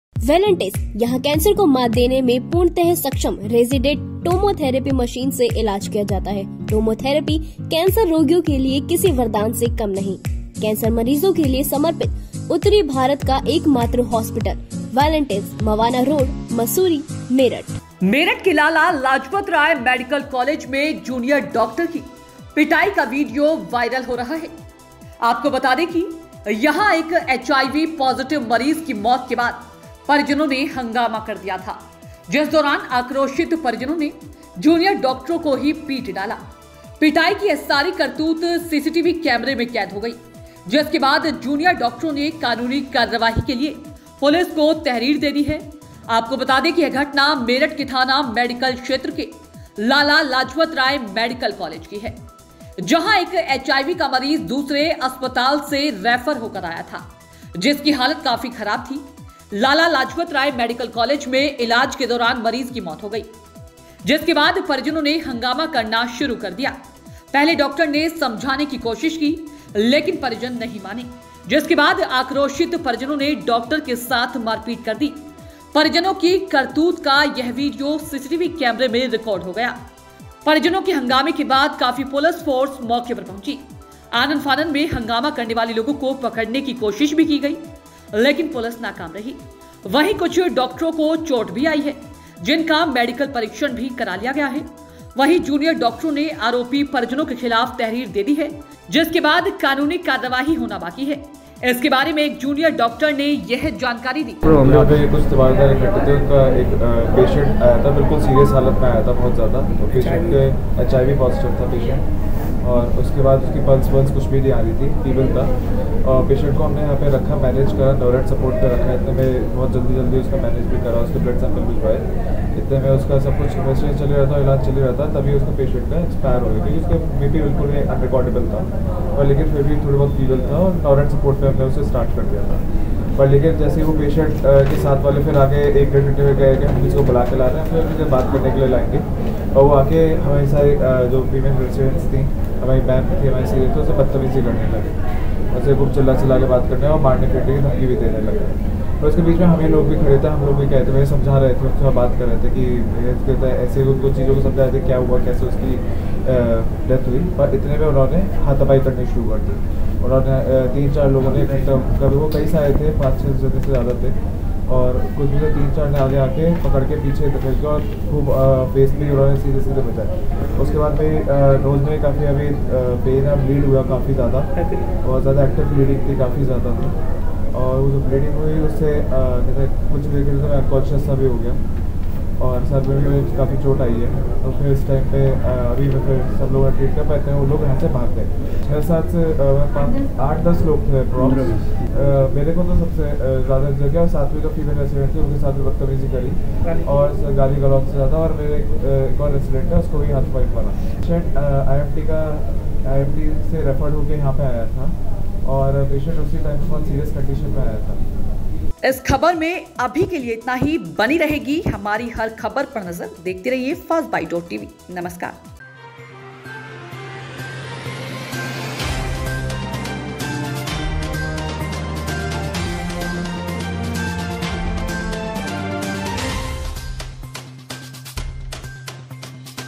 लाला लाजपत राय मेडिकल कालेज में डॉक्टर की पिटाई, वीडियो वायरल । Firstbyte.Tv
मेरठ के लाला लाजपत राय मेडिकल कॉलेज में जूनियर डॉक्टर की पिटाई का एक वीडियो वाइरल हो रहा है । बता दे की यहा एक एचआईवी पॉजिटिव मरीज की मौत के बाद परिजनों ने हंगामा कर दिया । जिस दौरान आक्रोशित परिजनों ने जूनियर डॉक्टरों को ही पीट डाला । पीटाई की यह सारी करतूत सीसीटीवी कैमरे में रिकॉर्ड हो गयी । जिसके बाद जूनियर डॉक्टरों ने कानूनी कार्रवाई के लिए पुलिस को तहरीर दे दी है । आपको बता दे की यह घटना मेरठ के थाना मेडिकल क्षेत्र के लाला लाजपत राय मेडिकल कॉलेज की है । जहां एक एचआईवी का मरीज़ दूसरे हॉस्पिटल से रेफर होकर आया था, जिसकी हालत काफी खराब थी । लाला लाजपत राय मेडिकल कॉलेज में इलाज के दौरान मरीज की मृत्यु हो गई । जिसके बाद परिजनों ने हंगामा करना शुरू कर दिया । पहले डॉक्टर ने समझाने की कोशिश की गई । लेकिन परिजन नहीं माने जिसके बाद आक्रोशित परिजनों में डॉक्टर के साथ मारपीट की । परिजनों की करतूत का यह वीडियो सीसीटीवी कैमरे में रिकॉर्ड हो गया । परिजनों के हंगामे के बाद काफी पुलिस फोर्स मौके पर पहुंची । आनन-फानन में हंगामा करने वाले लोगों को पकड़ने की कोशिश भी की गई । लेकिन पुलिस नाकाम रही । वहीं कुछ डॉक्टरों को चोटें भी आई हैं । जिनका मेडिकल परीक्षण भी करा लिया गया है । वहीं जूनियर डॉक्टरों ने आरोपी परिजनों के खिलाफ तहरीर दे दी है । जिसके बाद आप कानूनी कार्रवाई होना बाकी है । इस बारे में एक जूनियर डॉक्टर ने यह जानकारी दी